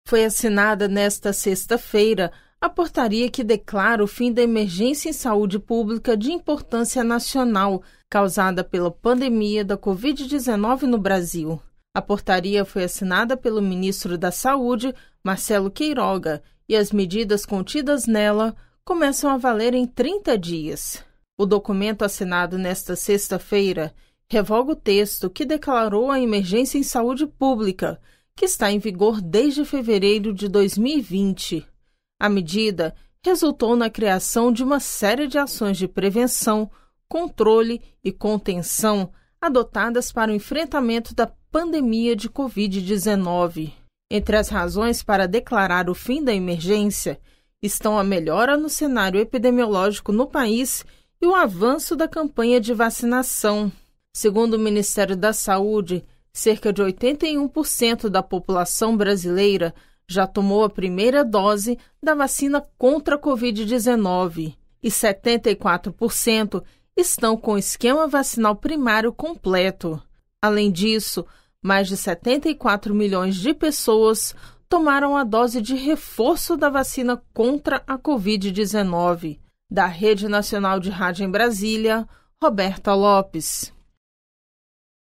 22/04/2022 - QUEIROGA ASSINA PORTARIA QUE COLOCA FIM NA EMERGÊNCIA EM SAÚDE PÚBLICA
Portaria assinada hoje (22) pelo ministro da Saúde, Marcelo Queiroga, declara o fim da Emergência em Saúde Pública de Importância Nacional, causada pela pandemia da Covid-19. Medidas começam a valer em 30 dias.

Foi assinada nesta sexta-feira (22) a portaria que declara o fim da Emergência em Saúde Pública de Importância Nacional, causada pela pandemia da Covid-19 no Brasil. A portaria foi assinada pelo ministro da Saúde, Marcelo Queiroga, e as medidas contidas nela começam a valer em 30 dias.

A portaria assinada nesta sexta-feira, revoga o texto que declarou a emergência em saúde pública, que está em vigor desde fevereiro de 2020. A medida resultou na criação de uma série de ações de prevenção, controle e contenção adotadas para o enfrentamento da pandemia de covid-19.

Entre as razões para declarar o fim da emergência estão a melhora no cenário epidemiológico no país e o avanço da campanha de vacinação.  Segundo o Ministério da Saúde, cerca de 81% da população brasileira já tomou a primeira dose da vacina contra a covid-19 e 74% estão com o esquema vacinal primário completo. Além disso, mais de 74 milhões de pessoas tomaram a dose de reforço.

Da Rede Nacional de Rádio, em Brasília, Roberta Lopes

registrado em